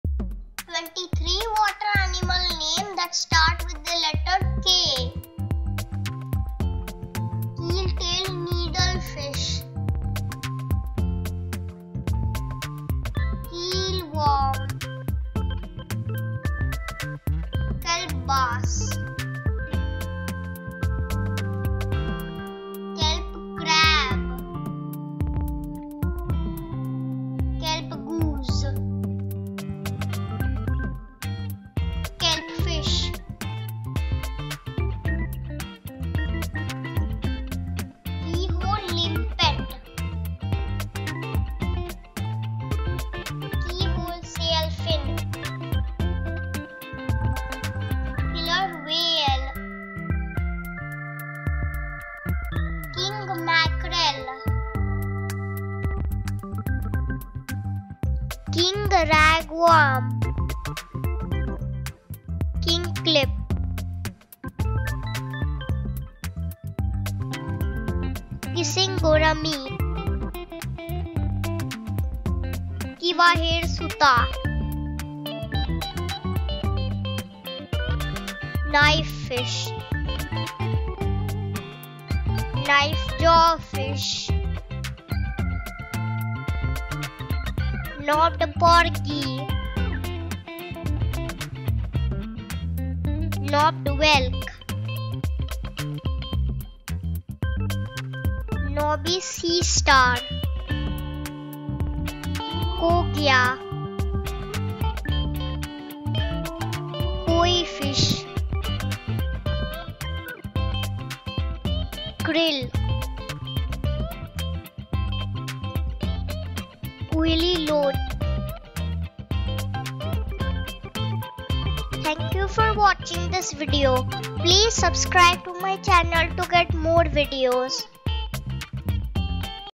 The ABCs of Aquatic Wonders-Part 11: 23 Fascinating Water Animal which names start with Letter “K”
Get ready for an underwater extravaganza! In this educational video, kids will embark on a thrilling journey through the depths of the sea to explore 23 captivating water animals whose names start with the letter ‘K’.

Join us now to improve your vocabulary and expand your knowledge of marine life and discover the wonders of the K-animal kingdom!

As kids watch, listen, and repeat the names of these captivating K-animals, their vocabulary will flourish. By combining the excitement of marine exploration with language acquisition, we create a dynamic educational environment that fosters both curiosity and linguistic development.

Join us now and watch as your little ones eagerly absorb knowledge about aquatic animals, simultaneously building their English vocabulary. Through this immersive experience, we aim to instil a lifelong love for learning and language. Dive in with us and let the adventure begin!

Video and Images Copyright:
Please be informed that all the names and images used in this video are for information and educational purpose only.